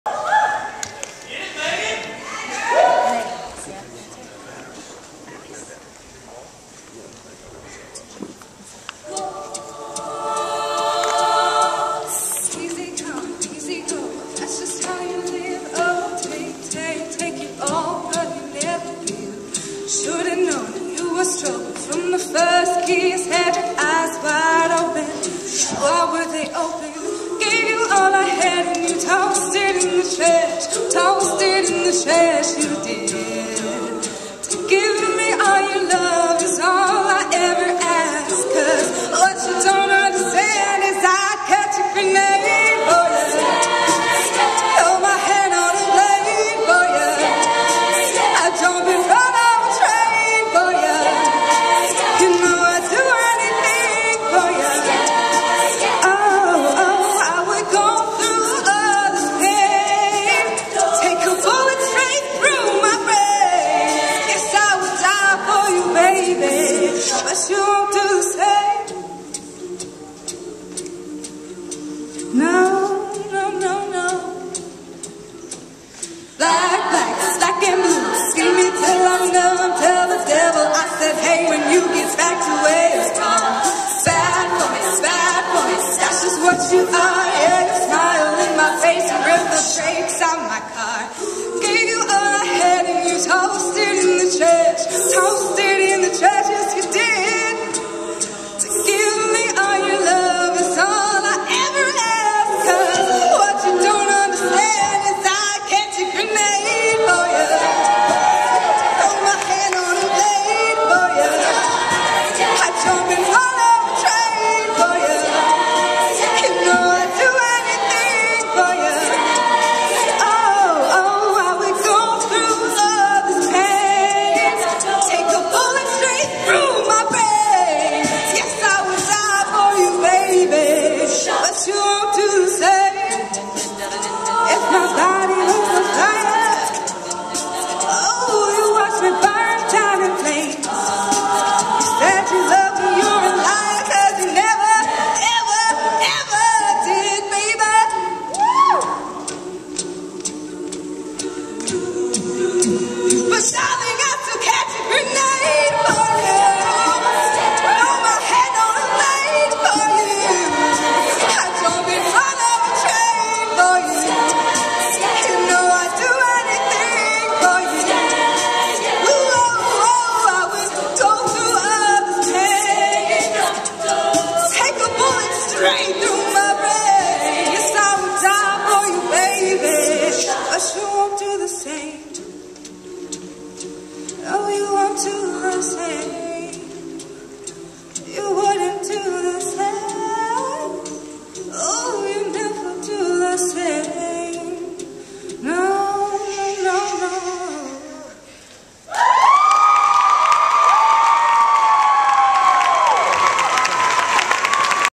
ICCA | Mid West Quarter Finals #4 | A Cub Bella Encore
If you know the song titles, send me the link to this video, I have not had the time to edit or review the songs.  Just posting up so that they can be reviewed.  Enjoy.  Had some camera/battery issues, so I did not get every song from every group.

BTW, KC On Cue is still auditioning for voices, we have a few positions left that we are trying to fill.